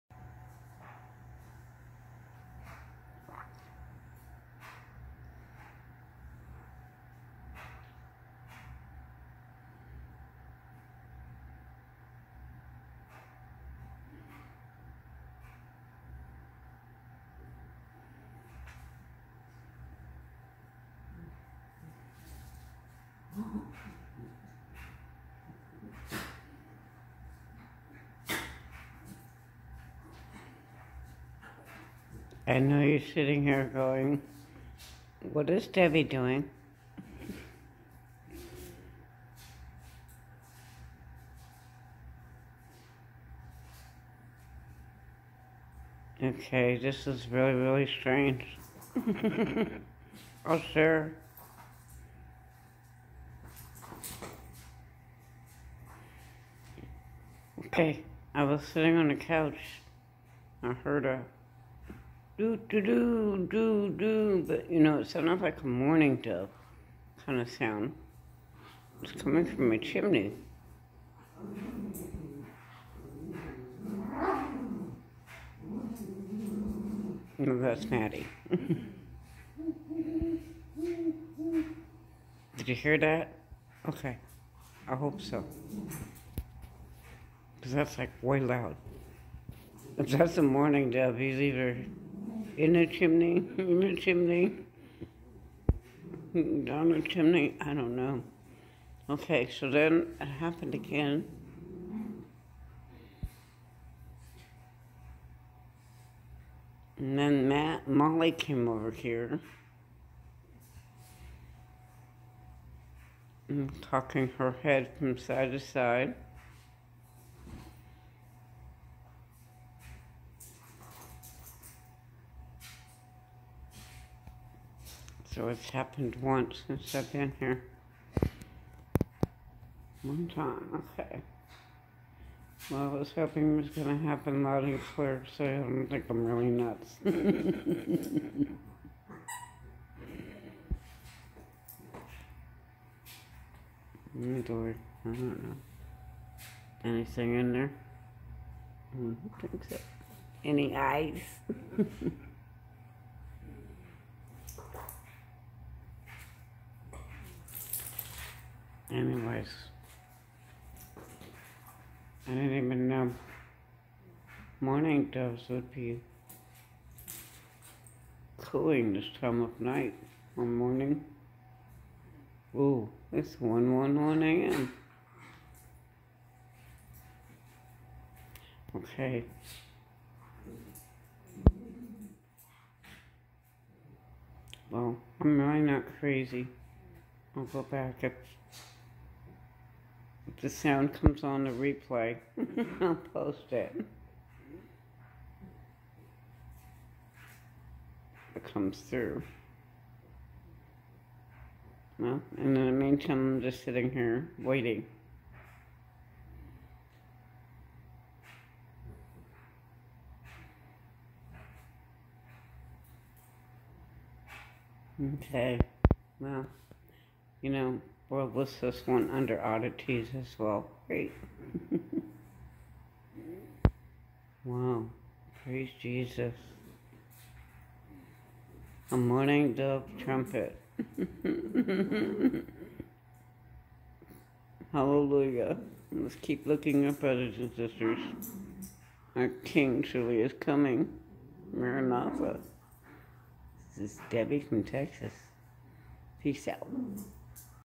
COME UP HITHER @119 🎺
Sounding half asleep, feel asleep on the couch. Sitting up I heard that sound loud and clear, like the cooing of a mourning dove, no garble sound it makes though, more like a trumpet with that rhythm. sounding like blasts, thought I was hearing thing's till Molly ran over there.

Trumpets are sounding!!!
Oh us crazy Christians!

Happened 4 time's,  you hear the 5th, hasn't happened since...

COME UP HITHER!!!